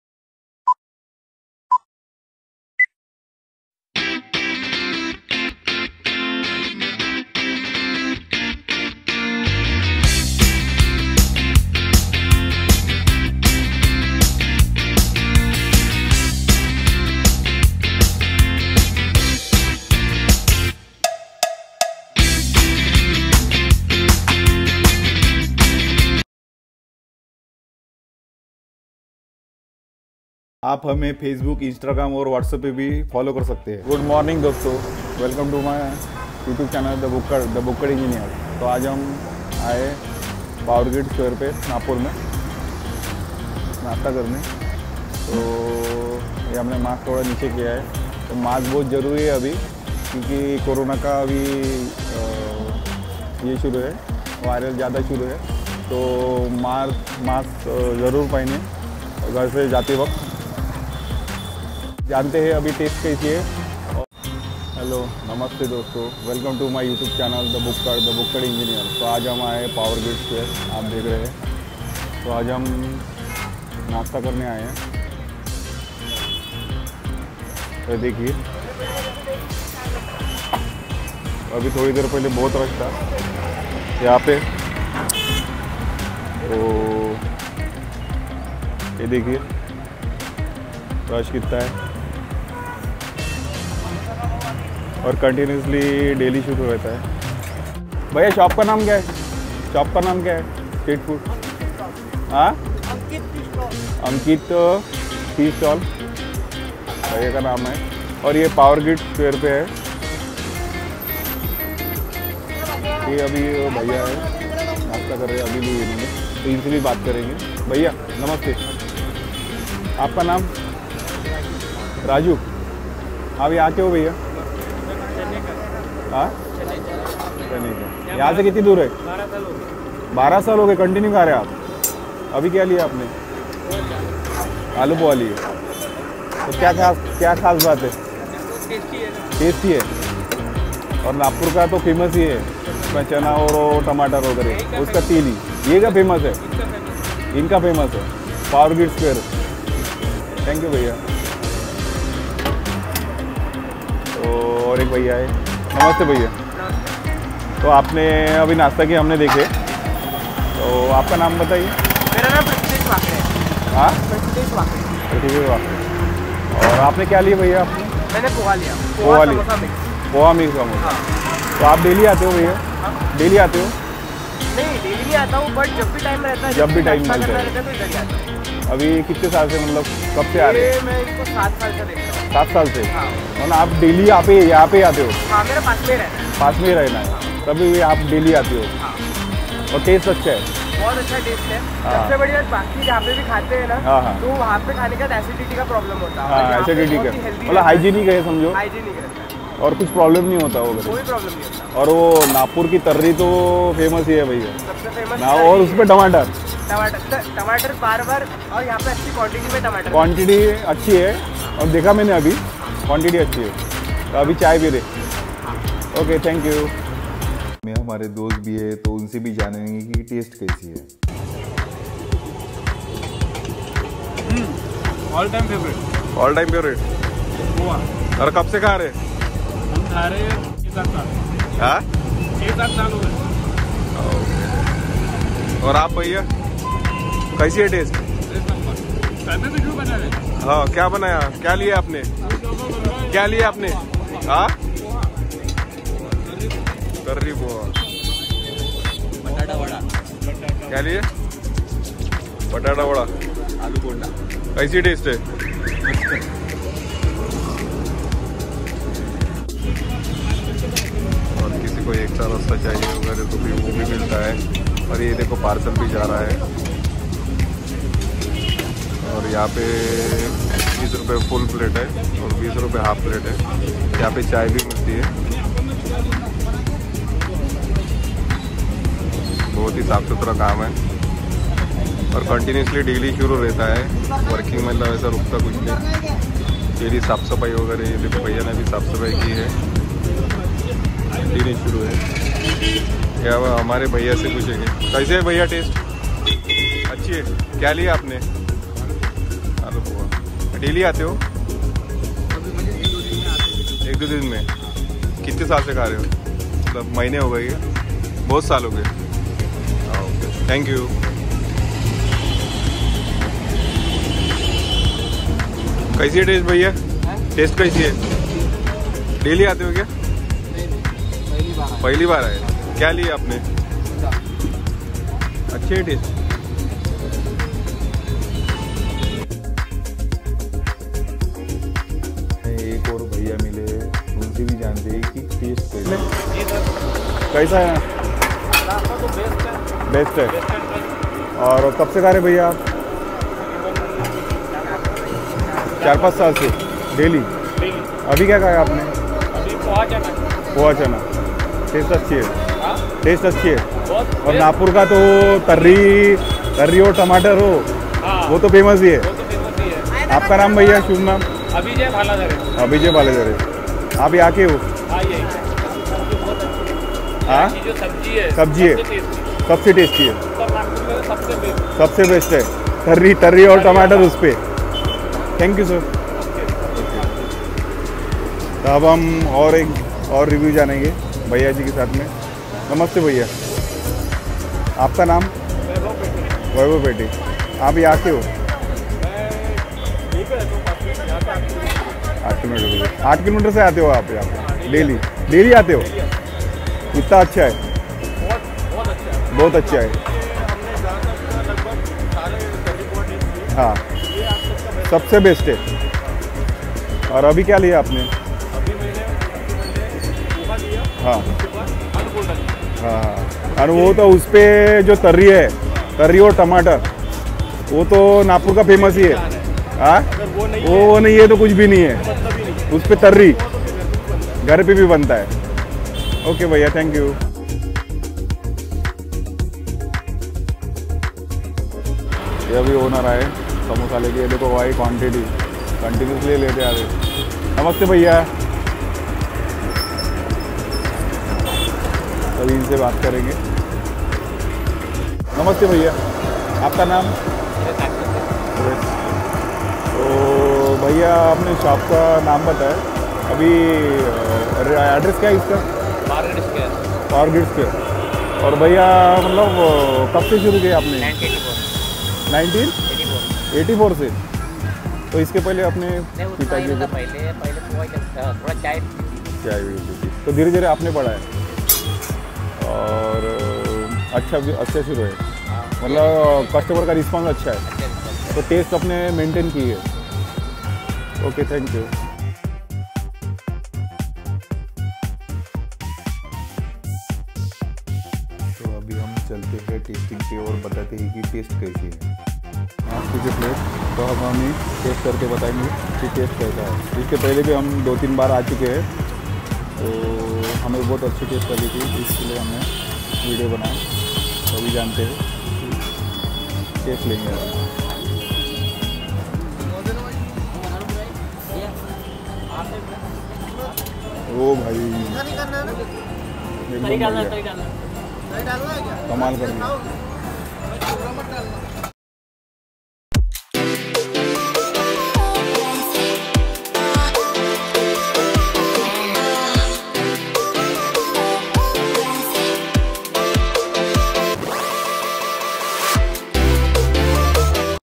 Tari Poha & Samosa Kadi in Nagpur // Nagpur Street Food // Best & Best
Ankit Tarri Poha & Tea Stall, POWer Grid Square, Nag Nari Road, Nagpur

🙏🙏Aur 2nd MY New Channel - AB Tech Channel
               aur nicha Doylya Link ko open & then like 👇🏻👇🏻 and share🤝👍